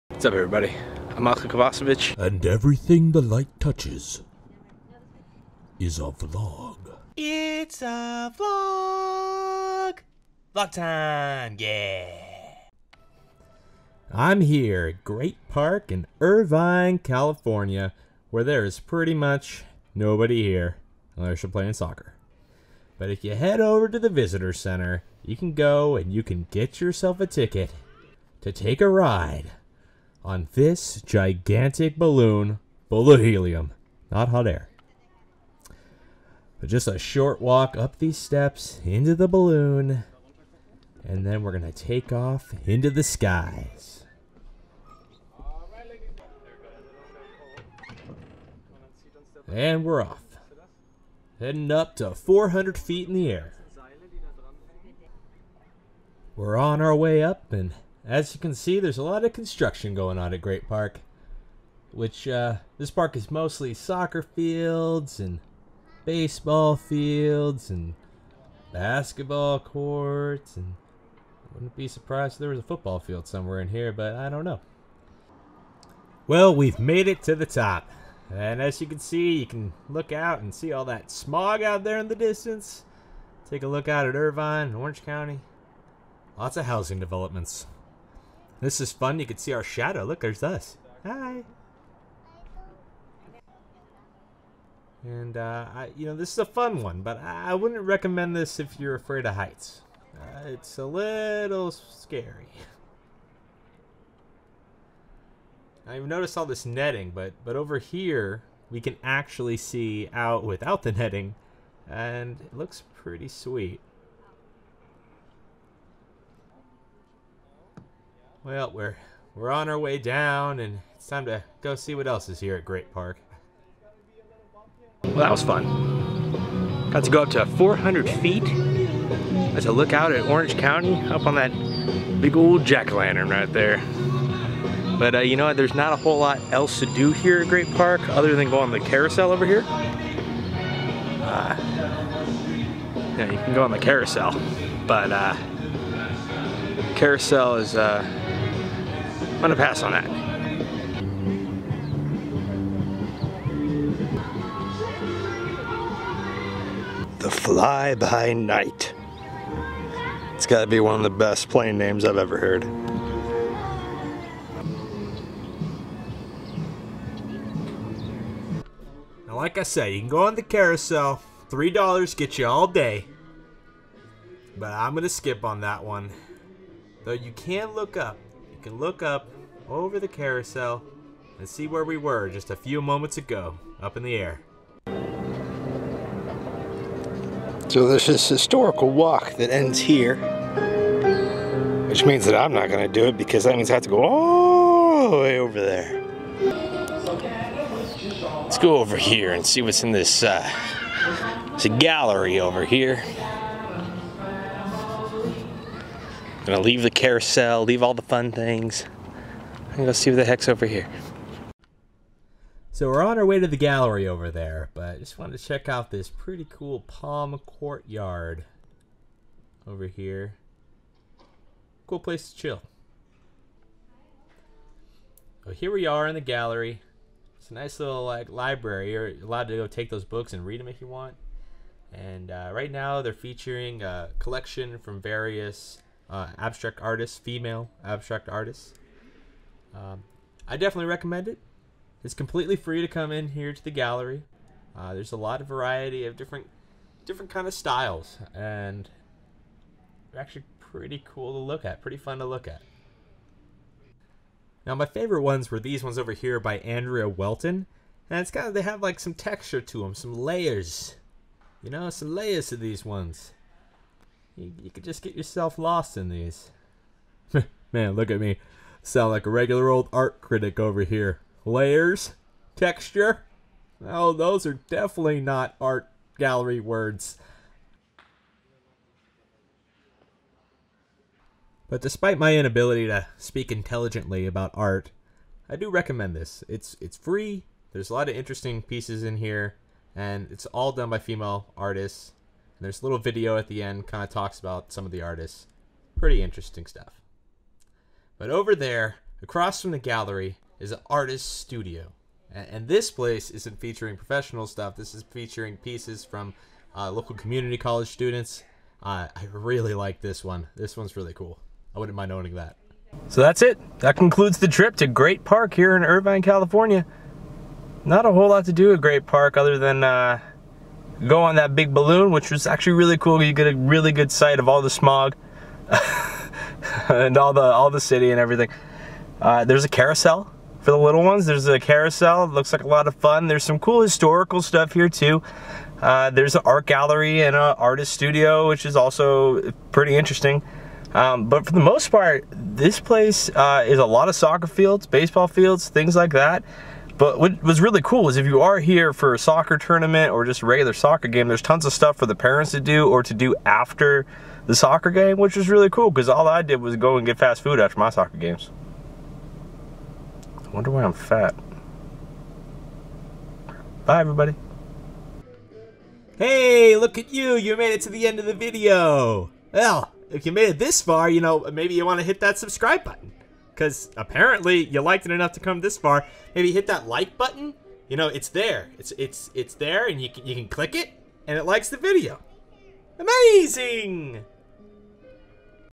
Great Park Balloon Ride And Gallery | It's A Vlog #5
Great Park in Irvine California has more to offer than just Soccer fields, Baseball fields, and Basketball courts. For just $10 (for adults) you can take a balloon ride up to 400ft in the air! There's also a Carousel and an Art Gallery. Join me as I take a ride on the Great Park Balloon, and explore what other offerings the park has for its visitors.

And for all my Disneyland pics